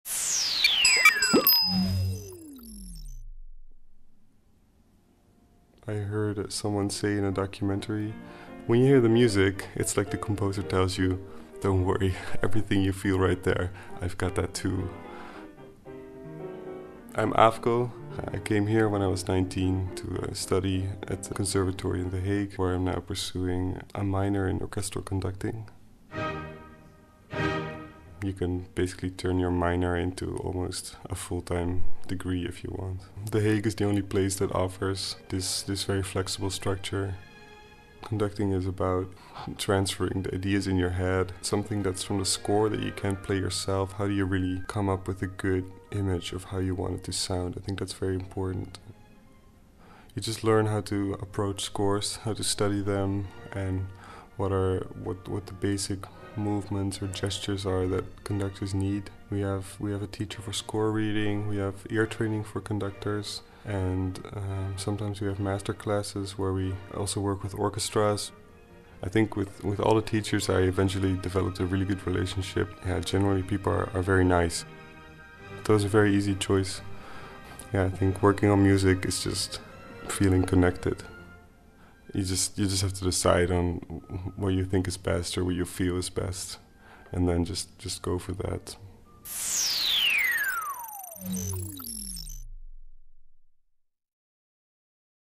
Be one of us: Open Day 2019 Royal Conservatoire - Aafko (conducting)
Aafko is studying Conducting and Piano through our Practicum Musicae programme. Practicum Musicae is a course for students of Leiden University with musical aspirations. Want to find out about this programme or our conducting programmes. Come be one of us during our Open Day on Saturday 26 January 2019.

Aafko studeert Directie en Piano via ons opleiding Practicum Musicae. Practicum Musicae is een gecombineerde opleiding voor studenten aan de Universiteit Leiden met muzikale aspiraties. Ben jij ook benieuwd naar deze opleiding of naar onze Directie-opleidingen. Be one of us en kom naar onze Open Dag op zaterdag 26 janauri 2019. Meer info